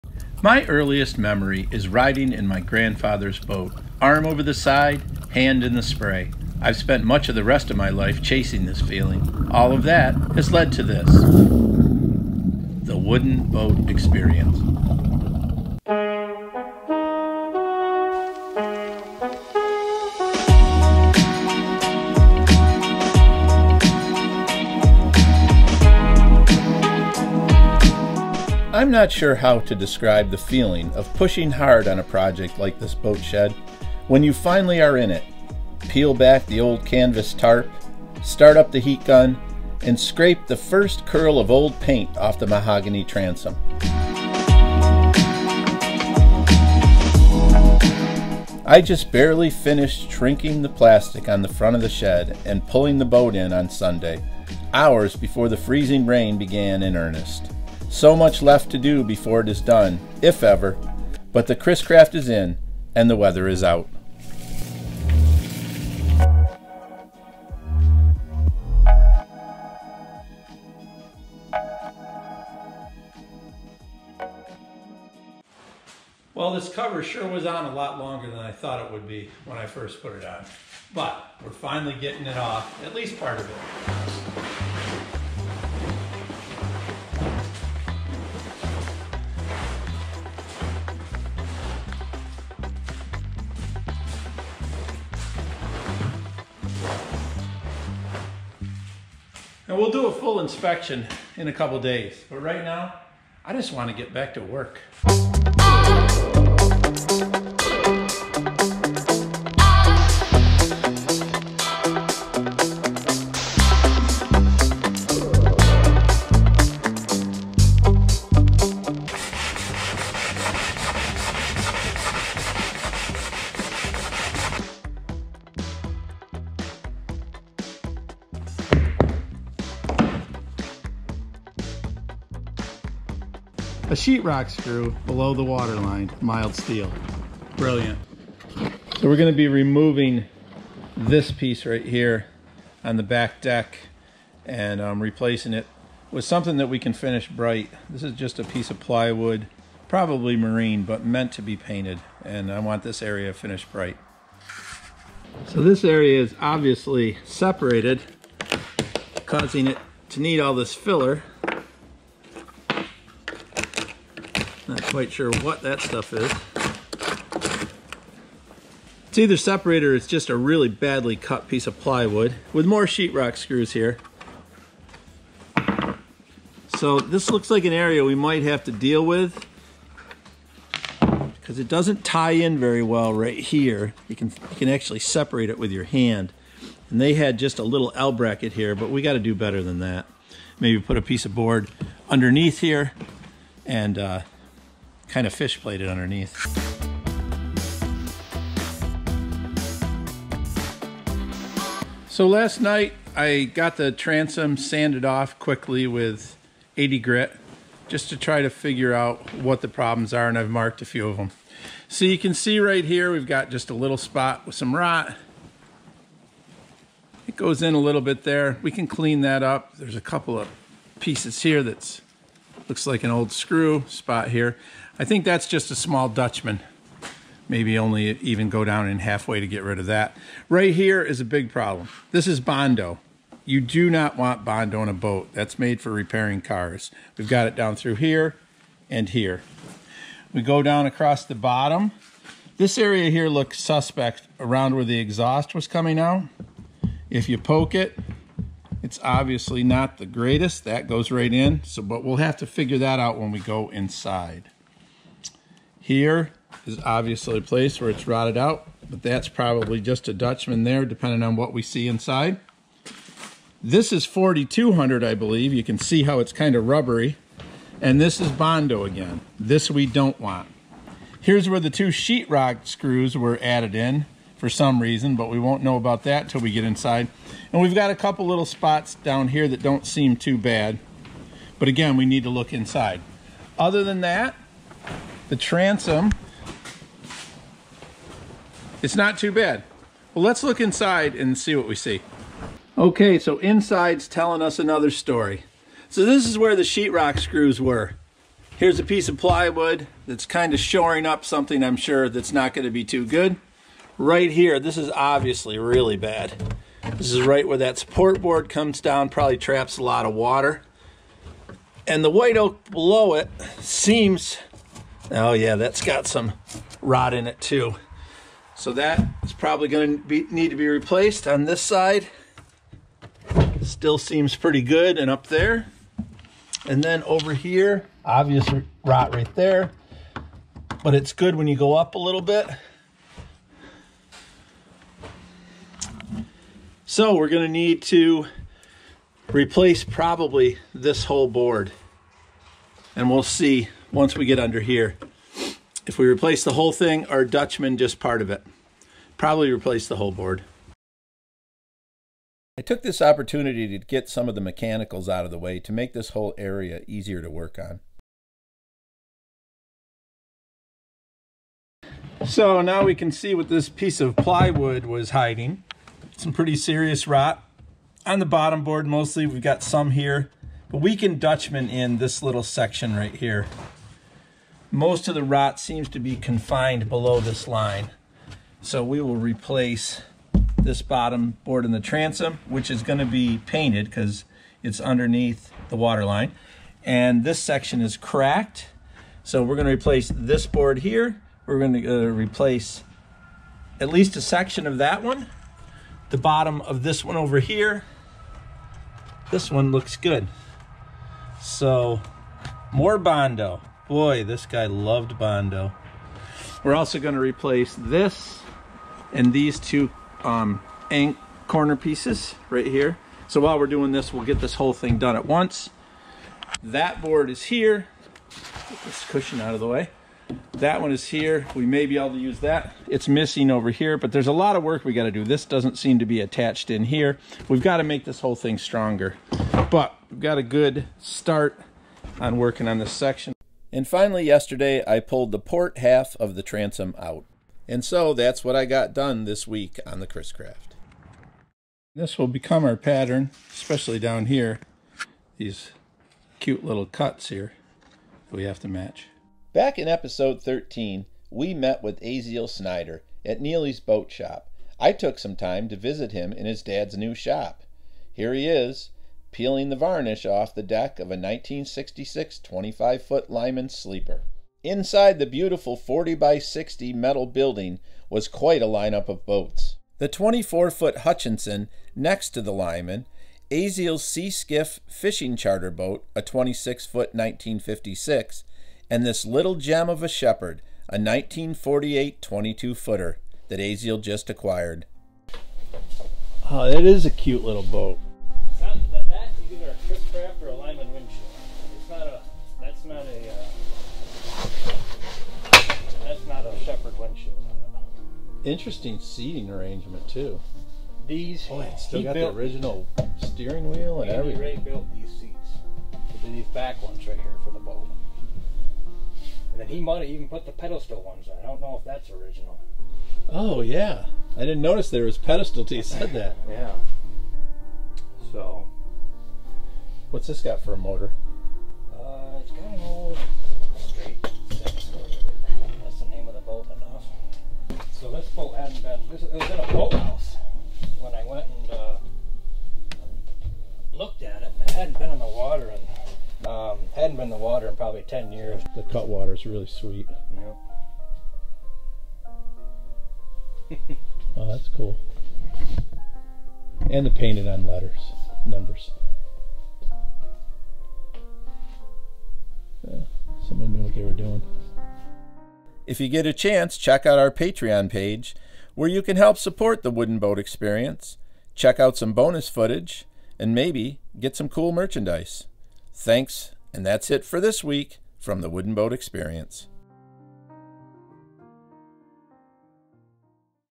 WBE S1E16 Chris Craft Restoration Begins!
Scott begins restoring the Chris Craft, visits a new shop building with a Lyman, Chris Craft, Shepard and of course, a Hutchinson.

Amazon Wish List

WBE tshirts are now available!
Messing About in Boats Tshirt:
WBE Stickers